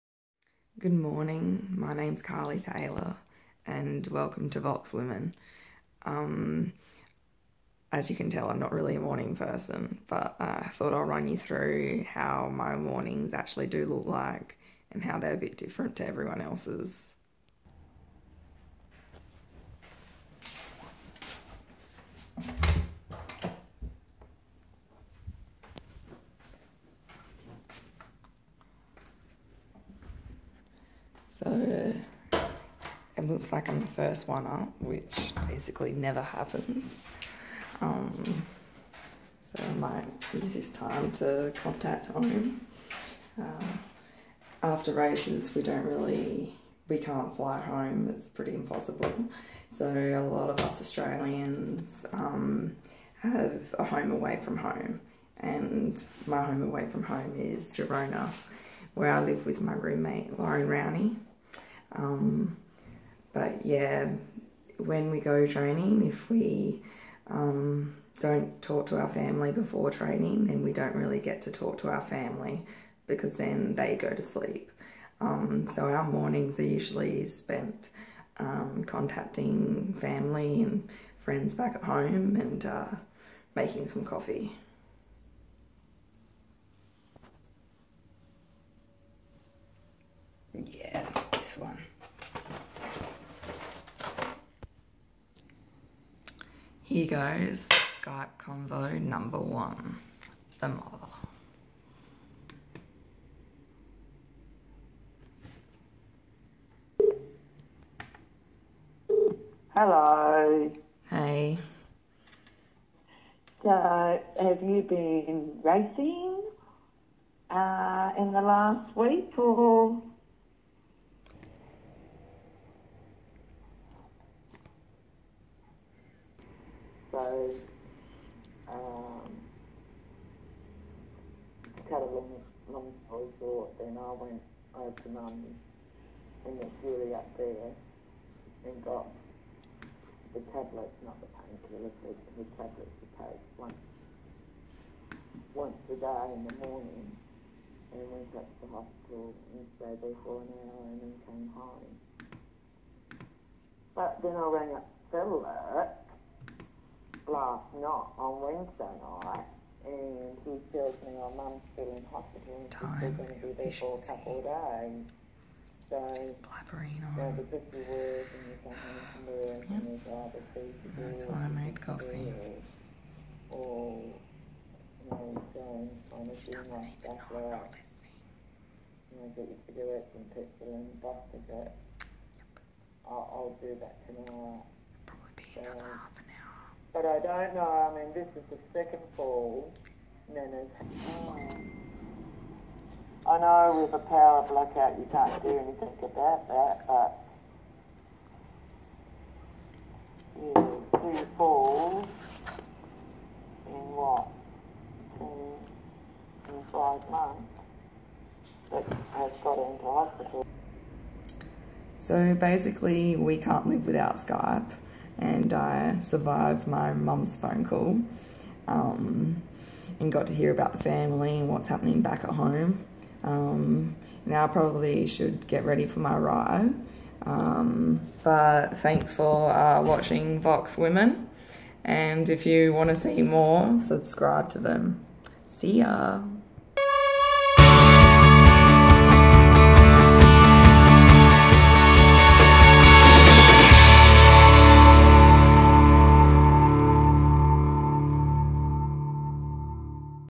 Carlee Taylor talks us through her morning routine
Follow Carlee on @carleetaylor1